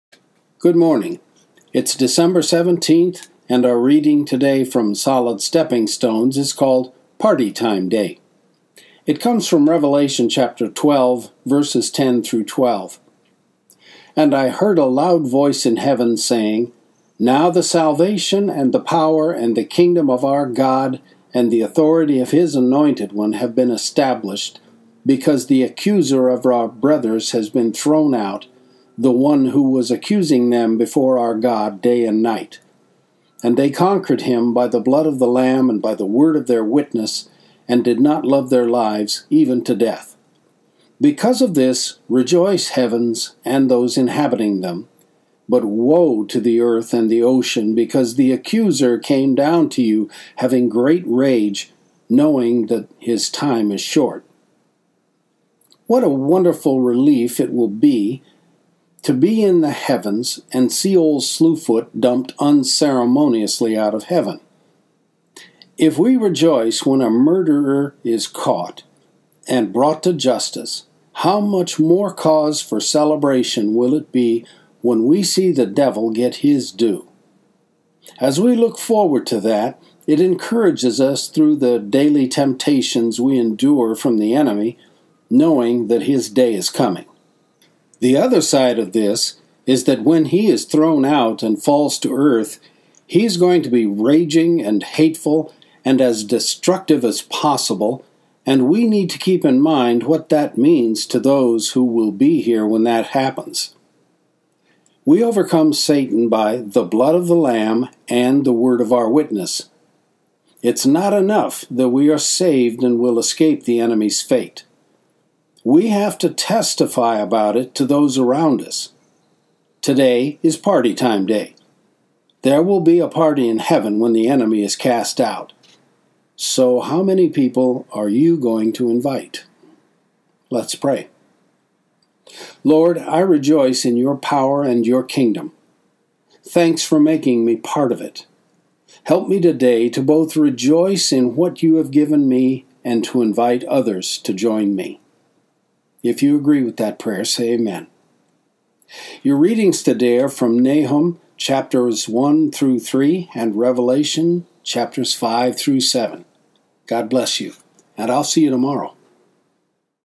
Solid Stepping Stones Dec 17 - Party Time Day
Meditation on Rev 12:10-12 from my book, "Solid Stepping Stones" Photos from the Mendenhall Glacier in Juneau, Alaska.
Look for my book on most major book outlets.